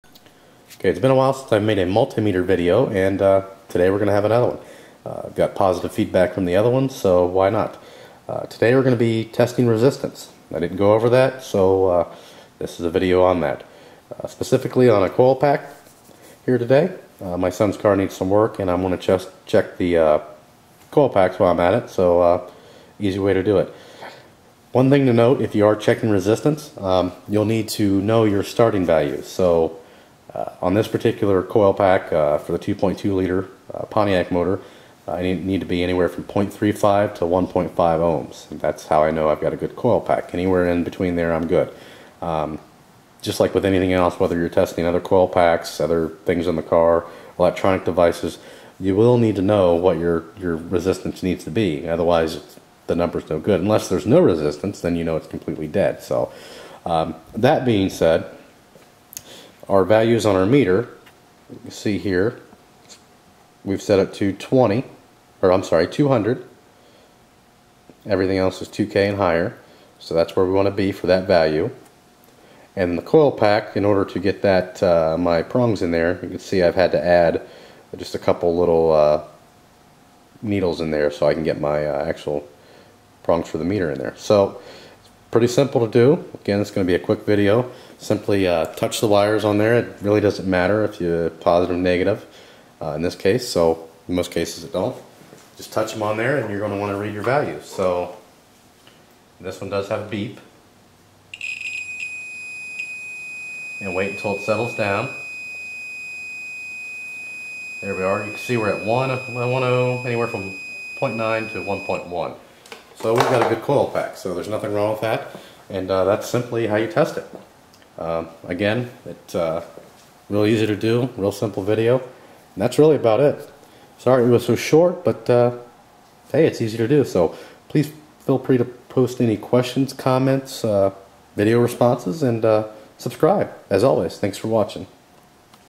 How to Check Resistance with a Multimeter. Automotive Coil Example
In this video learn how to check resistance using a multimeter. Here we use a spark plug coil as an example.